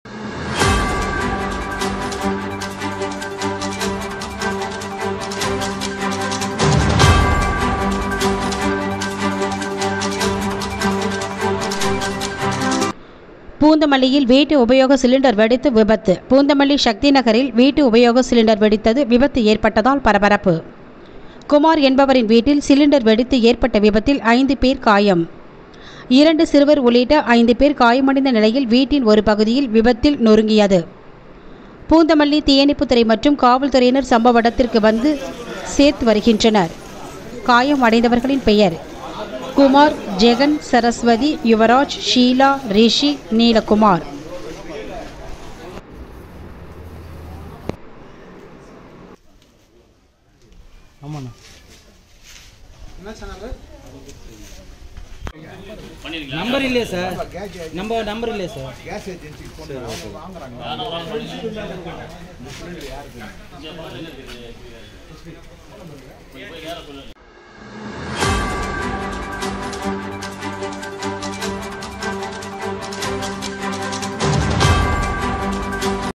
பூந்தமல்லியில் வீட்டு உபயோக சிலிண்டர் வெடித்து விபத்து
பூந்தமல்லி சக்தி நகரில் வீட்டு உபயோக சிலிண்டர் வெடித்தது விபத்து ஏற்பட்டதால் பரபரப்பு

குமார் என்பவரின் வீட்டில் சிலிண்டர் வெடித்து ஏற்பட்ட விபத்தில் ஐந்து பேர் காயம்

2 சிறுவர் உள்ளிட்ட 5 பேர் காயமடைந்த நிலையில் வீட்டின் ஒரு பகுதி விபத்தில் நெருங்கியது..

பூந்தமல்லி தீயணைப்பு துறை மற்றும் காவல்துறையினர் சம்பவ இடத்துக்கு வந்து சேர்த்து வருகின்றனர்

 காயமடைந்தவர்களின் பெயர்

குமார்52,
 ஜெகன்,45
சரஸ்வதி,64
யுவராஜ்,10,
ஷீலா,40,
ரிஷி,8
நீலக்குமார்58